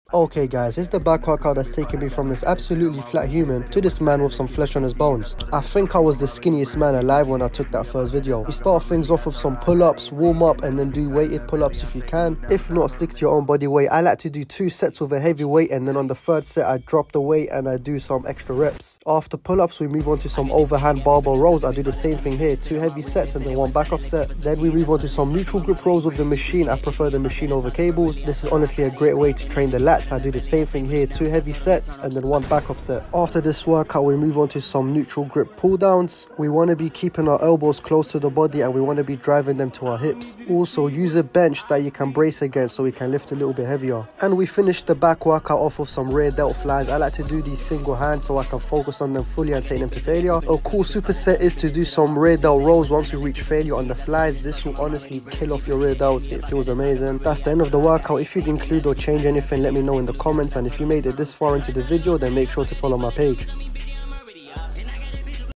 Struggling to Grow your Back? Try this 💪🏻
The back can be pretty tricky to grow, but not if you train them properly 💪🏻

Try this out and see if it works wonders for you too